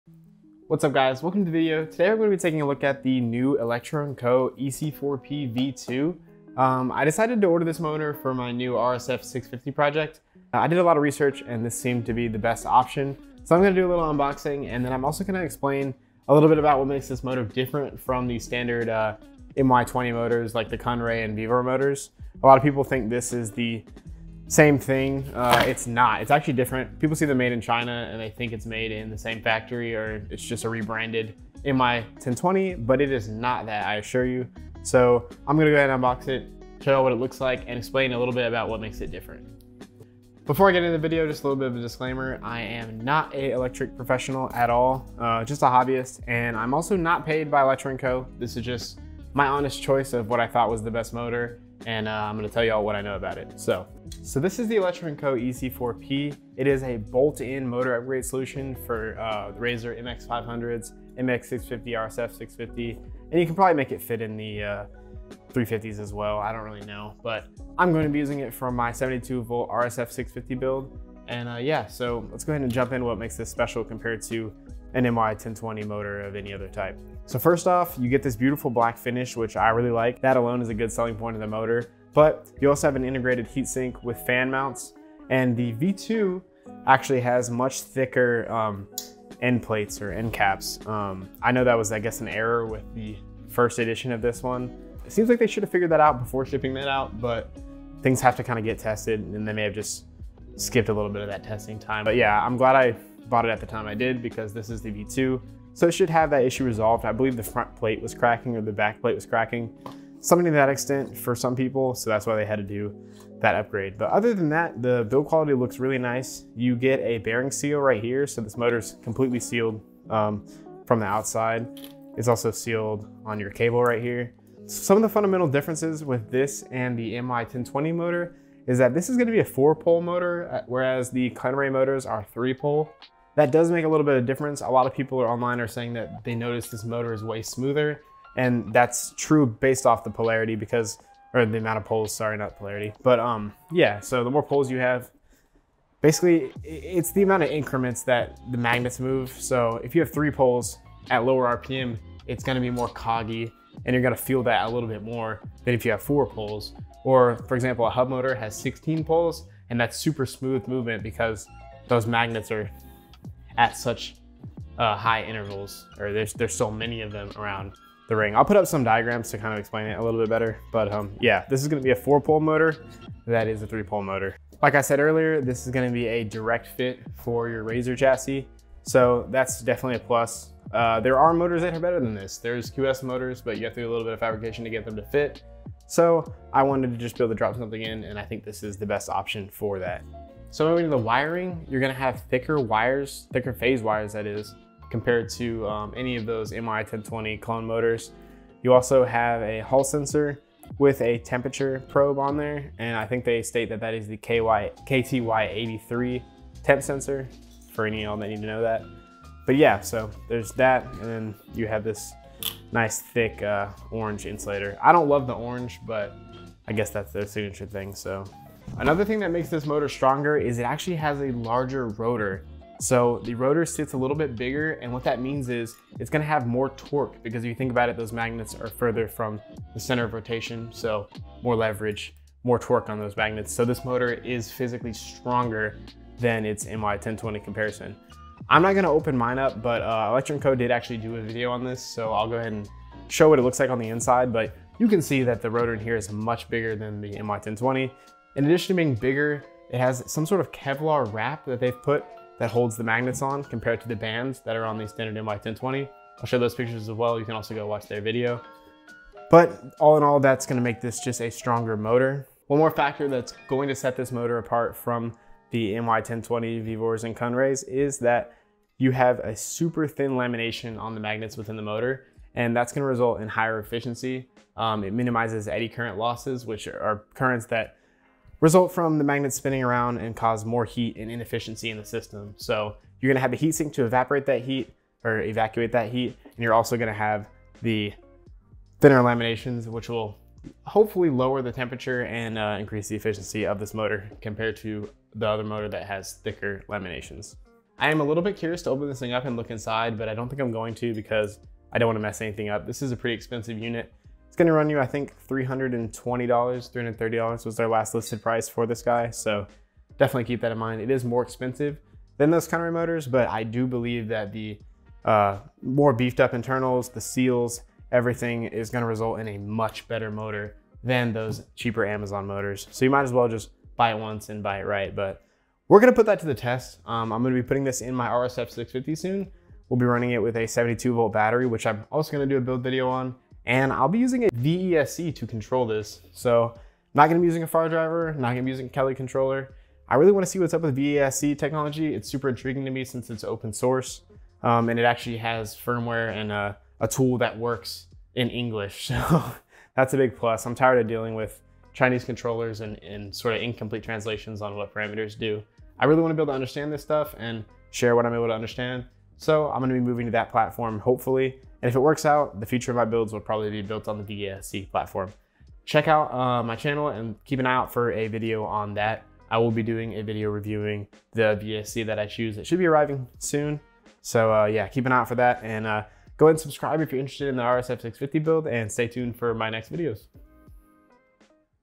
The BEST Razor Motor Upgrade? EC4P V2 Unboxing + Overview
The Electro And Co EC4P is a direct bolt in motor upgrade for Razor chassis such as MX500, MX650, RSF650, or anything else you can fit it in. Its form factor is the same as that of Kunray, Vevor, and knock off MY1020 equivalent motors. Its rated to support up to 14KW of power.